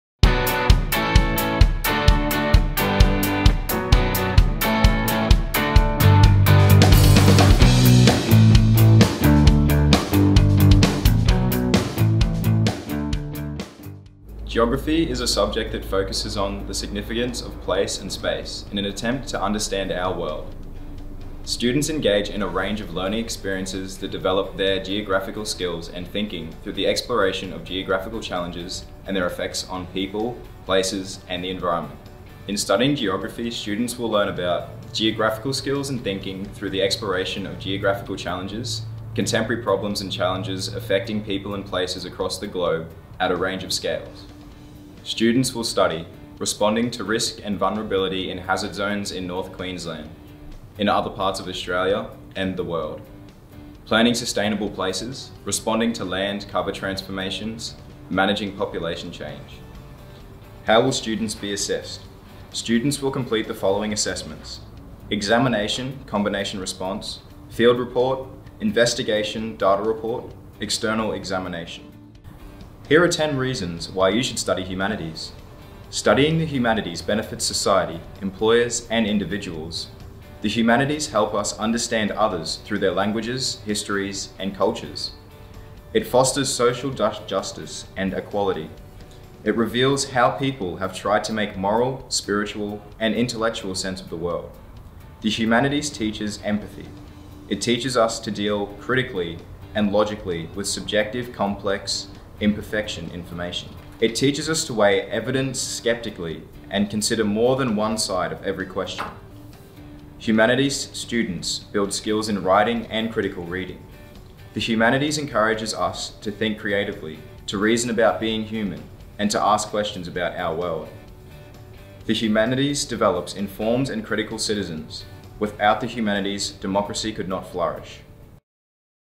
Humanities and Social Sciences | Geography | Year 10, 11 & 12
Geography teaches us about the significance of ‘place’ and ‘space’ in understanding our world. These two concepts are foundational to the
discipline, with the concepts of environment, interconnection, sustainability, scale and change building on this foundation. By observing and measuring spatial, environmental, economic, political, social and cultural factors,  geography provides a way of thinking about contemporary challenges and opportunities.